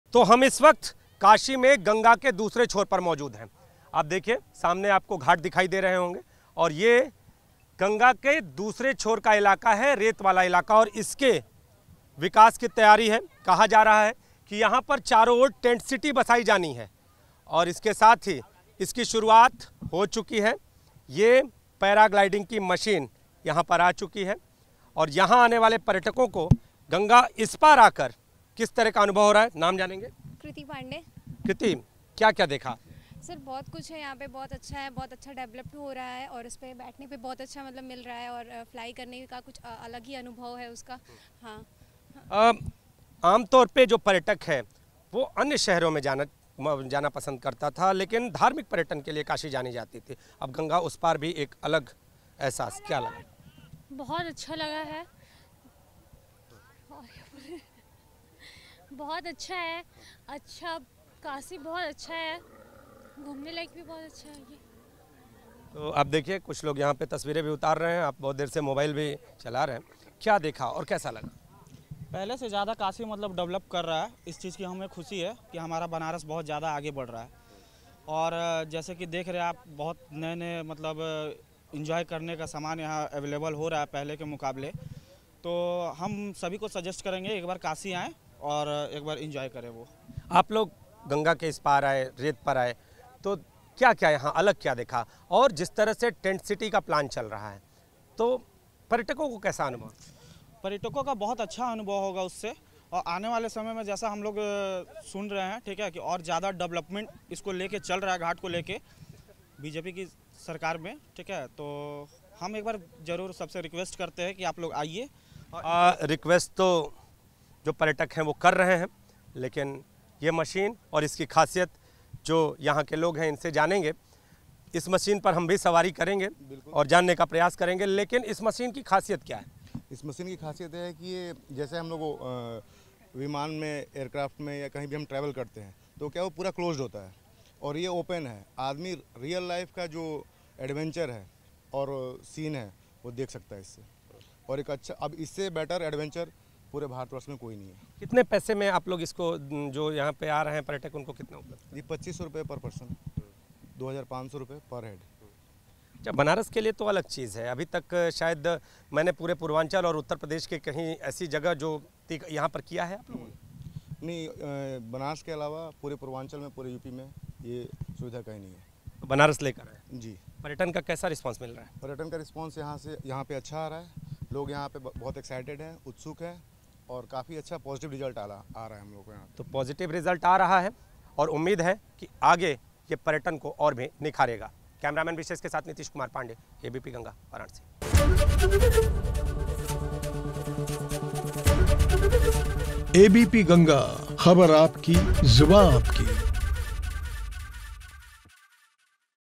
PM Modi के सपने को लगे पंख!, काशी के पर्यटन को धार दे रहा पैरा मोटर
PM Modi के सपने को लगे पंख, काशी के पर्यटन को धार दे रहा पैरा मोटर, क्या बोले लोग सुनिए...